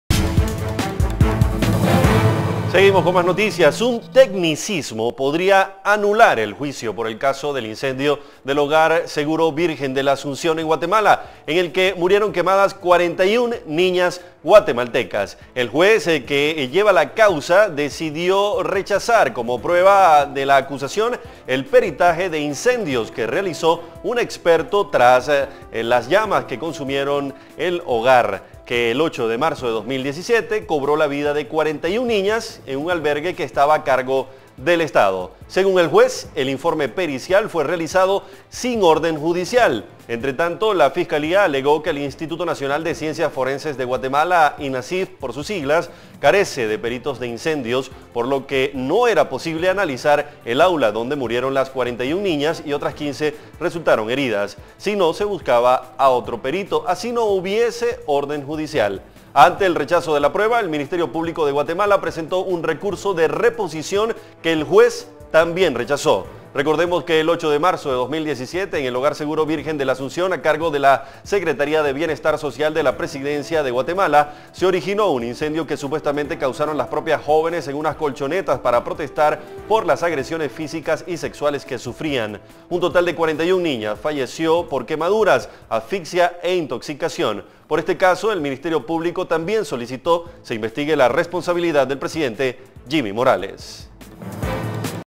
"Tecnicismo" podría anular juicio por incendio en el que murieron 41 niñas en Guatemala
Un "tecnicismo " podría anular el juicio por el caso del incendio del Hogar Seguro Virgen de la Asunción, en el que murieron quemadas 41 niñas guatemaltecas.

El juez que lleva la causa decidió rechazar como prueba de la acusación el peritaje de incendios que realizó un experto tras el incendio, que el 8 de marzo de 2017 se cobró la vida de 41 niñas en un albergue que estaba a cargo del Estado. Según el juez, el informe pericial fue realizado sin orden judicial. Entretanto, la fiscalía alegó que el Instituto Nacional de Ciencias Forenses de Guatemala, INACIF, carece de peritos de incendios.

Ante el rechazo de la prueba, el Ministerio Público de Guatemala presentó un recurso de reposición que el juez también rechazó.

El 8 de marzo del 2017, en el Hogar Seguro Virgen de la Asunción, a cargo de la Secretaría de Bienestar Social de la Presidencia de Guatemala, se originó un incendio que supuestamente causaron las propias jóvenes en unas colchonetas para protestar por las agresiones físicas y sexuales que sufrían. Un total de 41 niñas fallecieron por quemaduras, asfixias e intoxicaciones.

Por este caso el Ministerio Público también solicitó se investigue la responsabilidad del presidente Jimmy Morales.